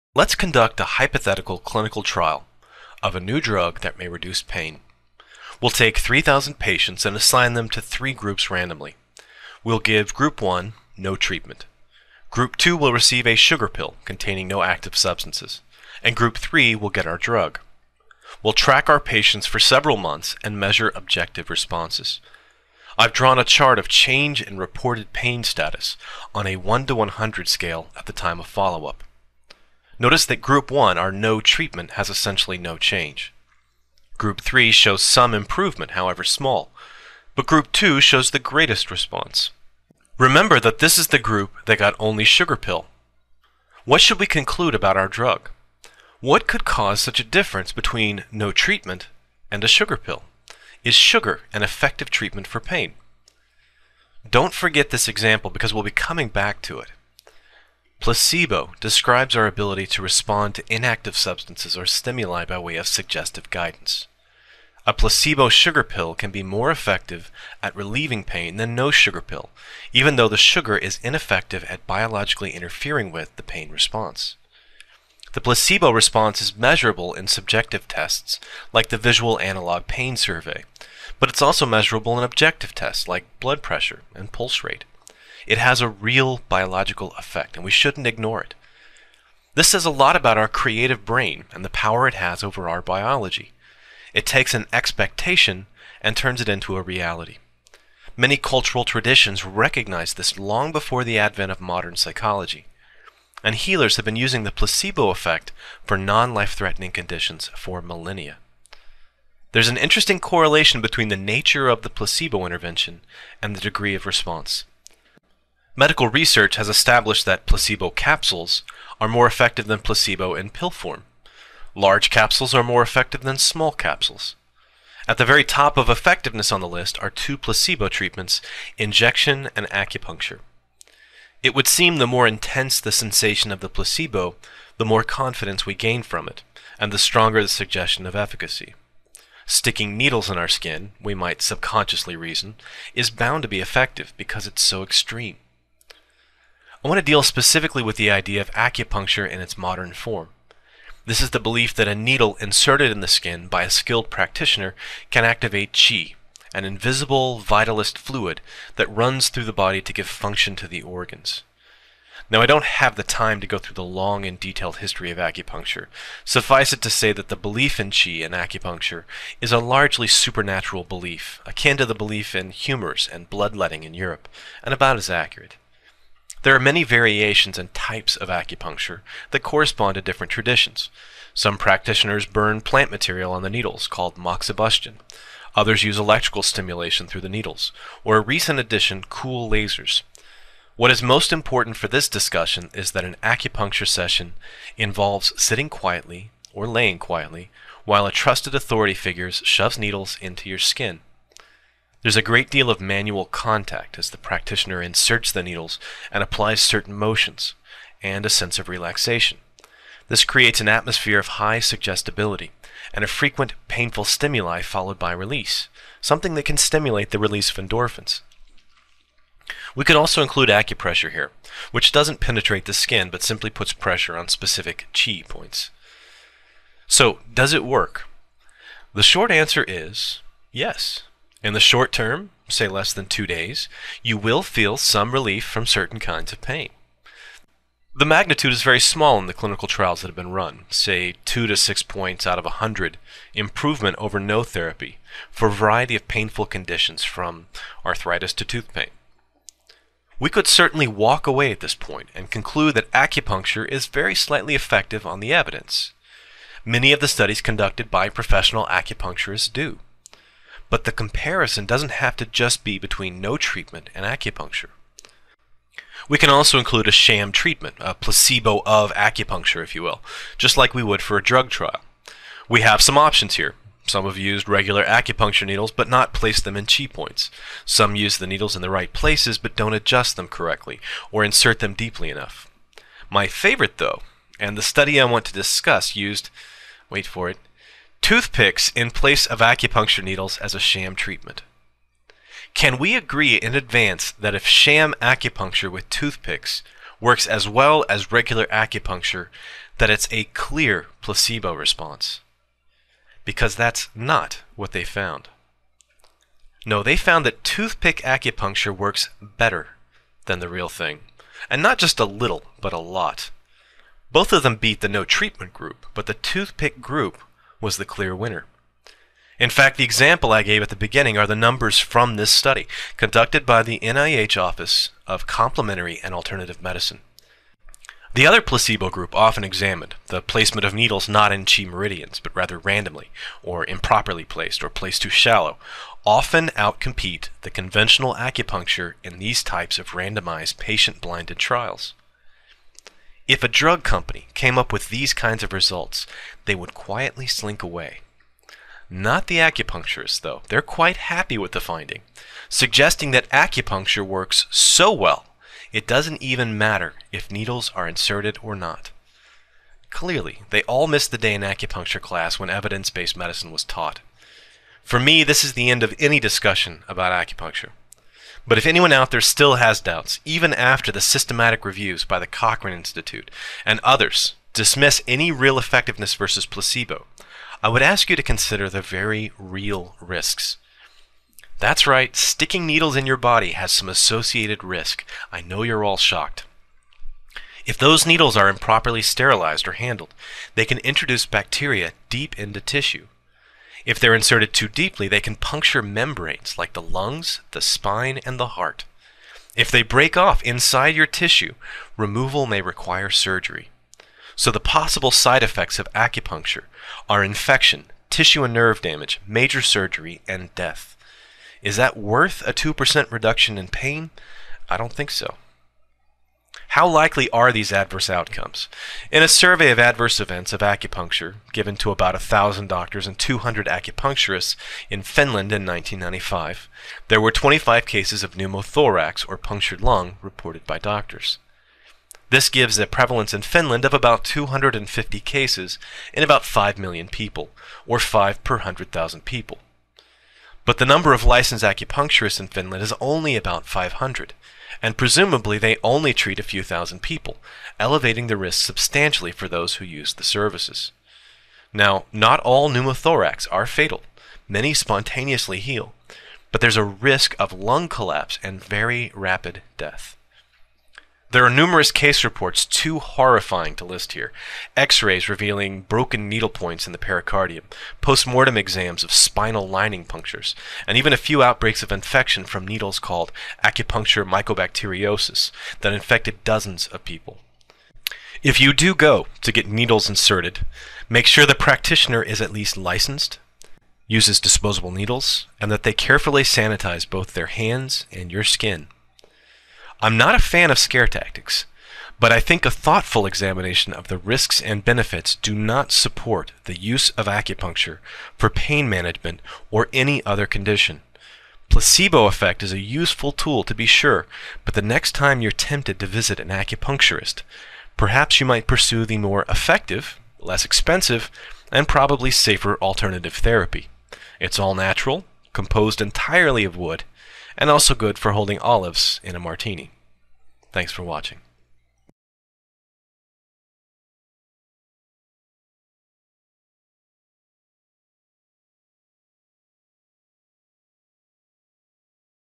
Βελονισμός
"Είναι ο βελονισμός αποτελεσματικός στη θεραπεία πόνου ή παθήσεων; Θα σας δώσω την επιστημονική απάντηση σε αυτό το ερώτημα, και θα εξηγήσω γιατί δεν συστήνω τον βελονισμό για οποιαδήποτε κατάσταση."

A few notes, and some references:
1. Toothpick acupuncture does NOT involve inserting the toothpick in the skin. It was merely placed against the skin in a needle guide-tube to give a sharp sensation, then removed.

2. In the USA, acupuncturists are required to meet sterile standards, use one-use-only needles or autoclaved (sterilized) needles, and must be licensed to practice.

3. I'm not really addressing the acupressure and non-insertive traditions of some Japanese acupuncture forms. Certainly these are similar to non-insertive sham treatments, but it should be noted that the placement of fingers or toothpicks is not important for the effect.

References:
1. BMJ 2009;338:a3115
Acupuncture treatment for pain: systematic review of randomised clinical trials with acupuncture, placebo acupuncture, and no acupuncture groups

2. Arch Intern Med. 2009;169(9):858-866
A Randomized Trial Comparing Acupuncture, Simulated Acupuncture, and Usual Care for Chronic Low Back Pain

3. Clin Med 2006;6:381--6
Systematic review of systematic reviews of acupuncture published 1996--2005

4. Lancet. 1995 Jun 17;345(8964):1576.
Adverse effects of acupuncture.